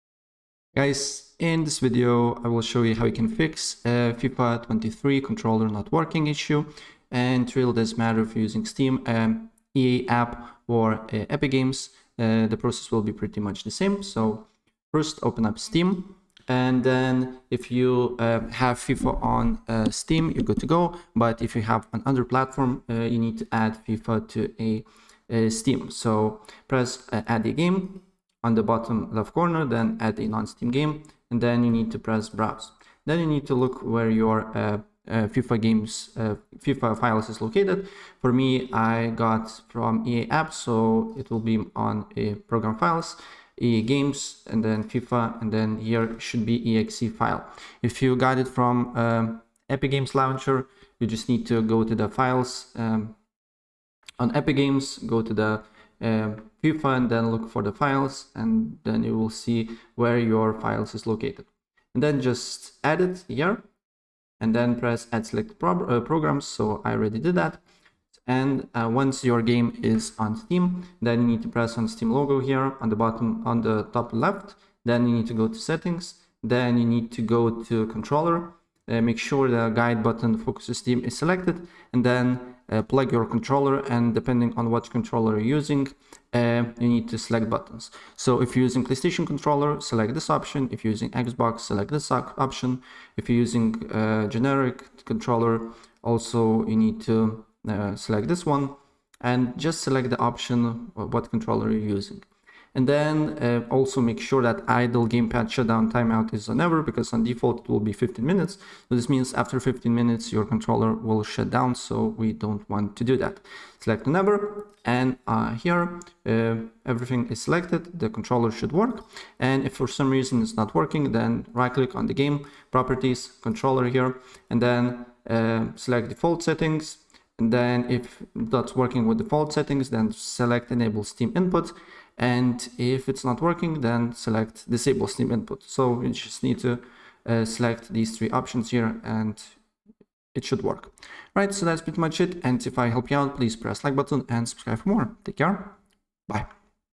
Fix Controller Not Working on FIFA 23 PC | Steam, EA App or Epic Games
In this video I will show you Fix Controller Not Working on FIFA 23 PC It's really easy and it will take you less than a minute to do it!